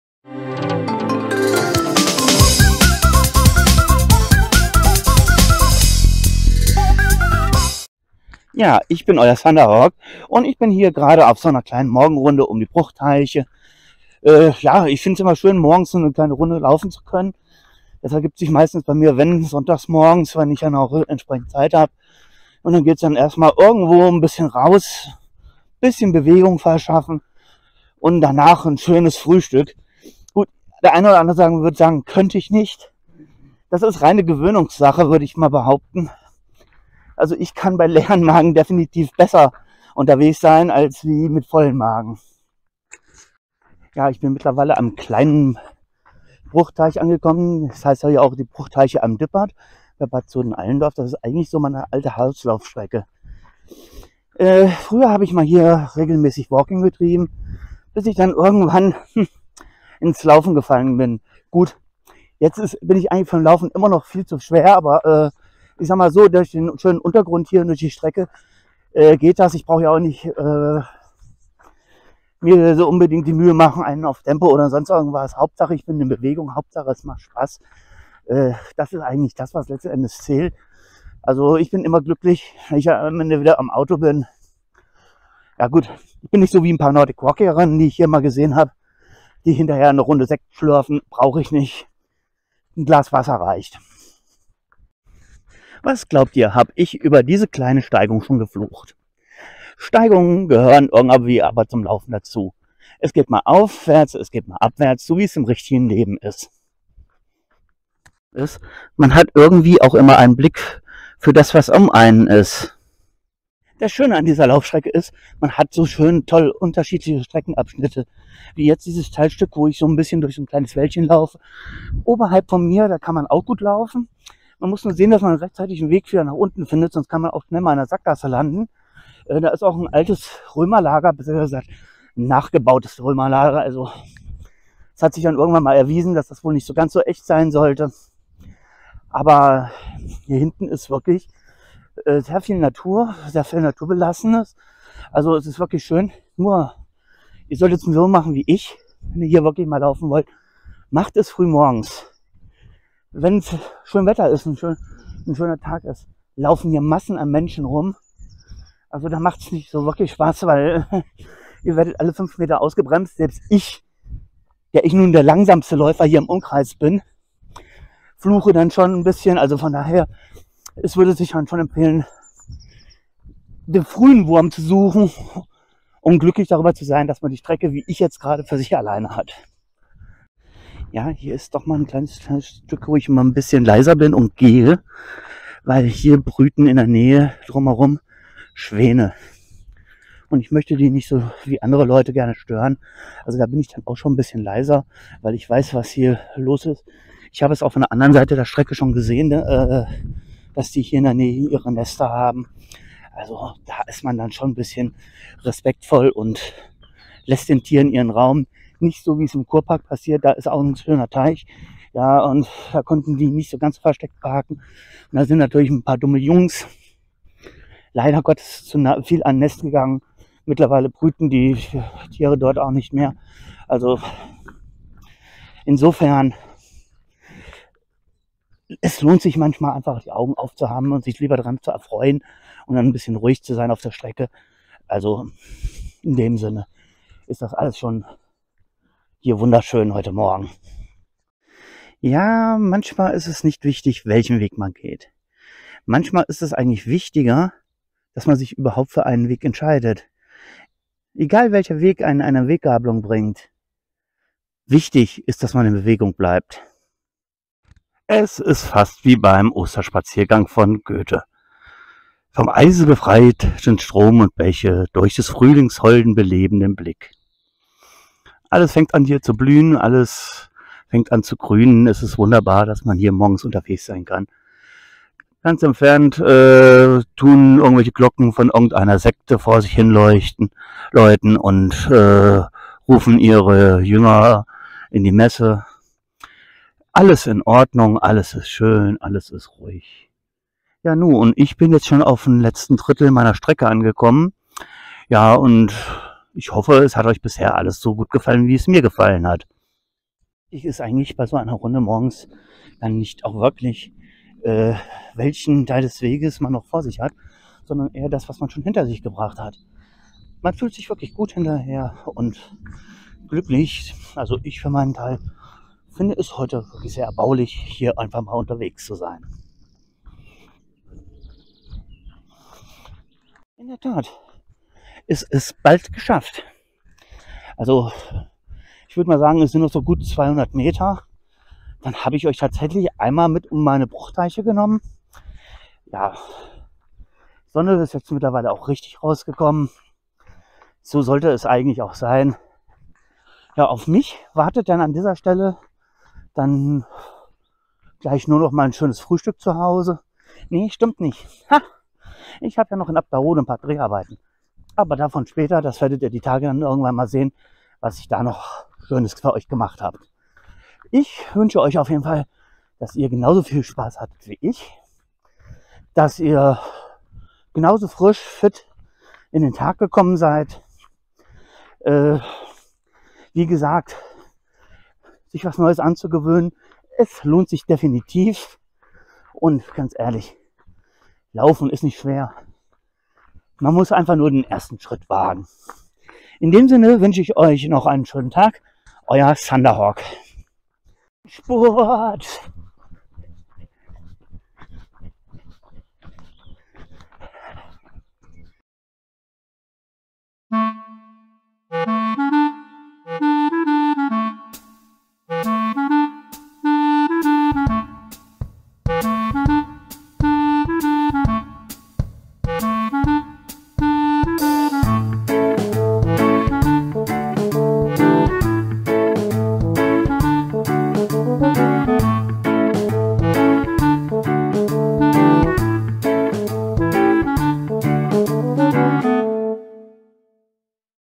Morgenlauf an den Bruchteichen
Heute morgen, schönstes Wetter und ich habe mir gedacht: Geh doch mal an den Bruchteichen laufen. Und was macht man so unter Freunden? Man nimmt die Freunde mit. Beim Laufen ist ein kleines Video entstanden. Schaut doch einfach mal rein.